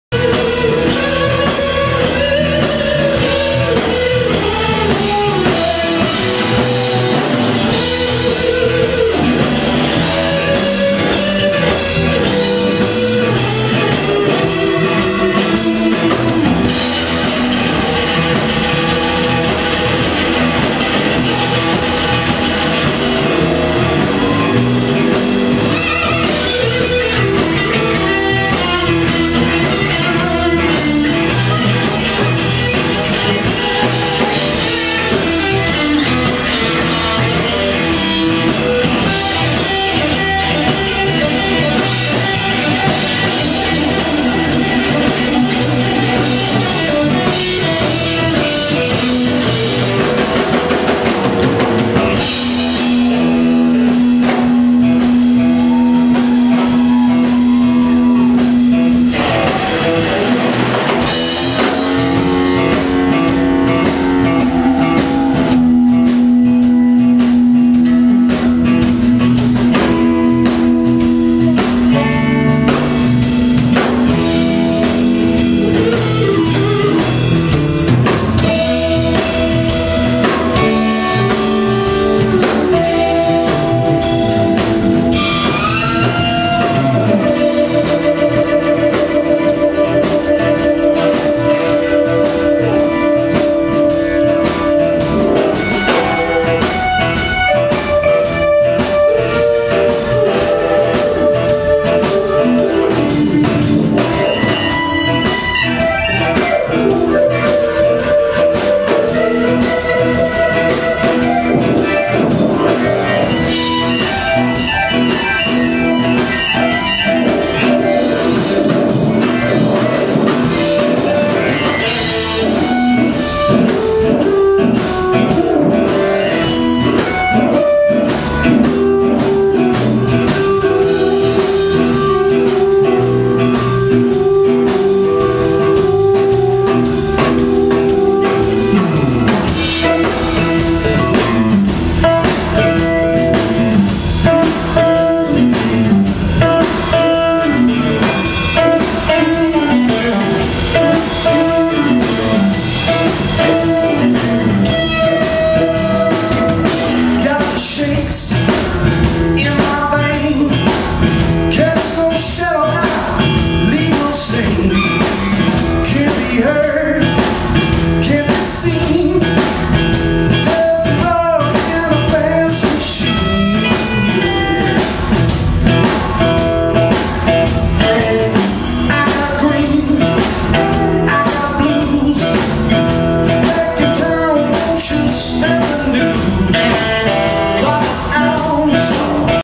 Jason Elmore and Jim Suhler
Encore for Jim's set at The Kessler Theater on December 27, 2010. The damn battery ran out before the end of the set. Freakin' technology.